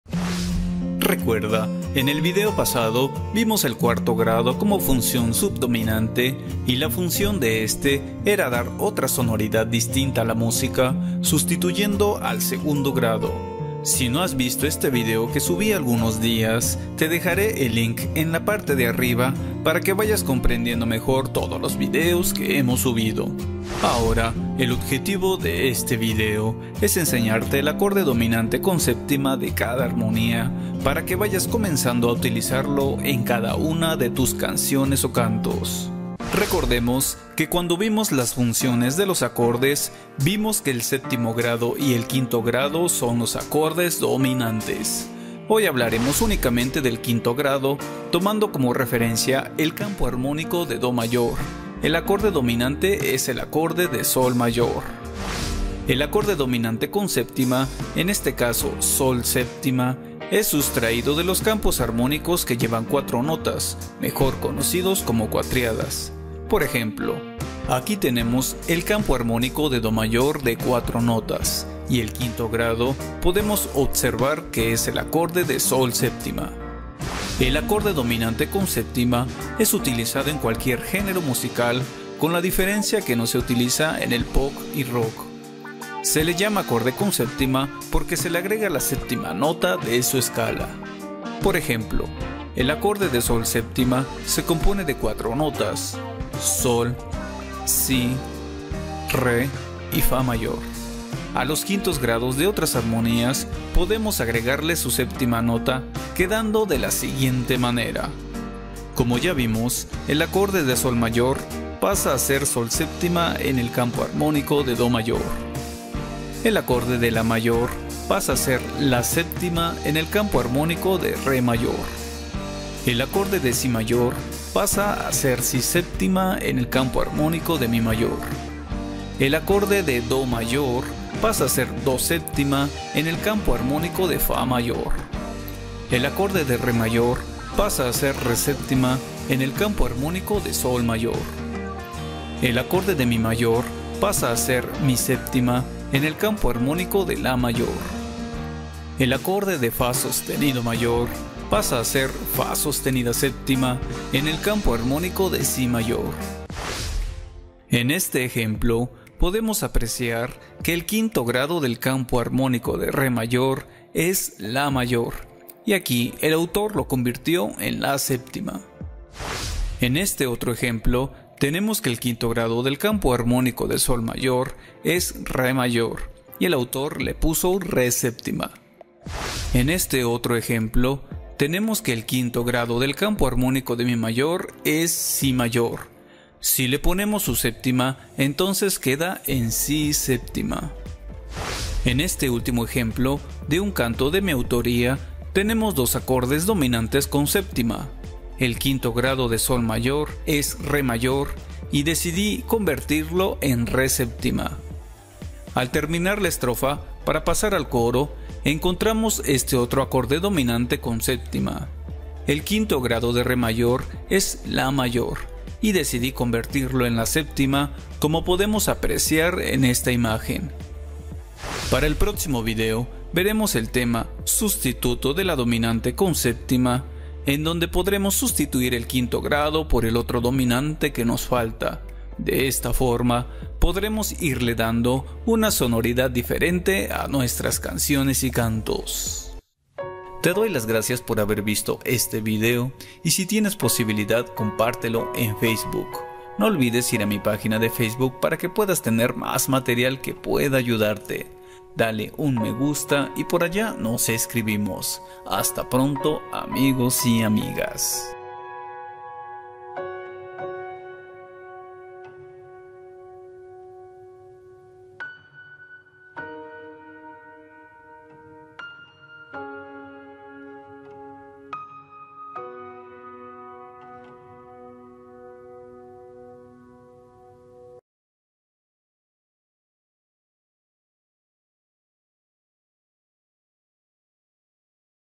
Acorde Dominante con 7ma
El objetivo de este video, es enseñarte el Acorde Dominante con 7ma de cada armonía, para que vayas comenzando a utilizarlo en cada una de tus canciones o cantos.